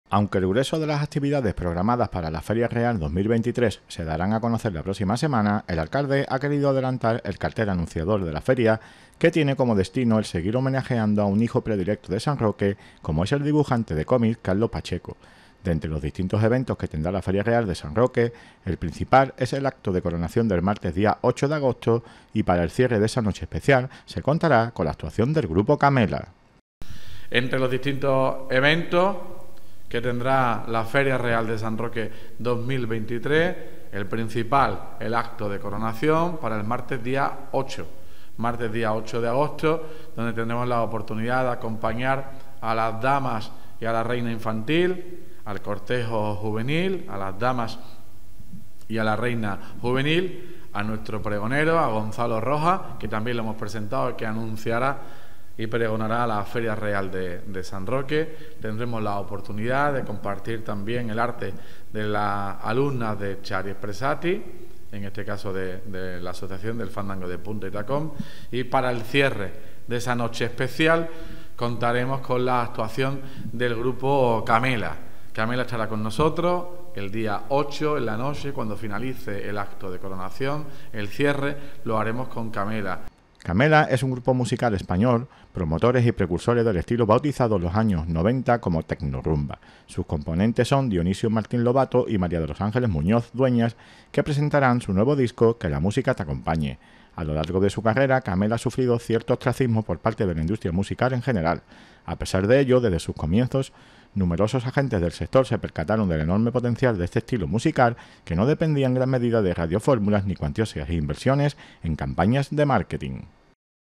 NOTICIA PRESENTACIÓN CARTEL Y ACTUACIONES CORONACIÓN FERIA SAN ROQUE 27 JULIO 2023
El alcalde de San Roque, Juan Carlos Ruiz Boix, y la teniente de alcalde de Fiestas, María Collado, han presentado el cartel y las actuaciones en la Coronación de la Feria Real de San Roque. El cartel es un homenaje a Carlos Pacheco, y en la Coronación actuarán Camela, las alumnas de Chari Expressati, y el en pregón de Gonzalo Rojas